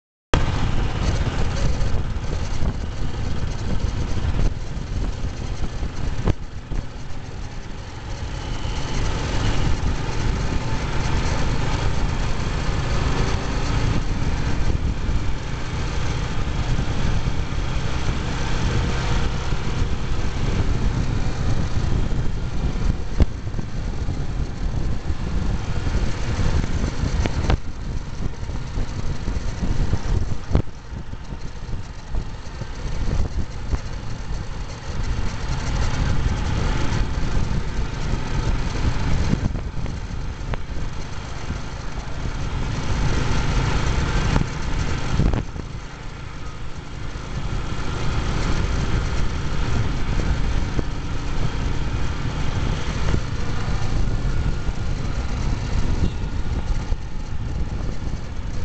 MANAN PATEL - TOP SPEED RUN AT 160 KMPH ON THE NH8 ON MY ROYAL ENFIELD MACHISMO AVL 350
SPEEDING ON THE NATIONAL HIGHWAY NO 8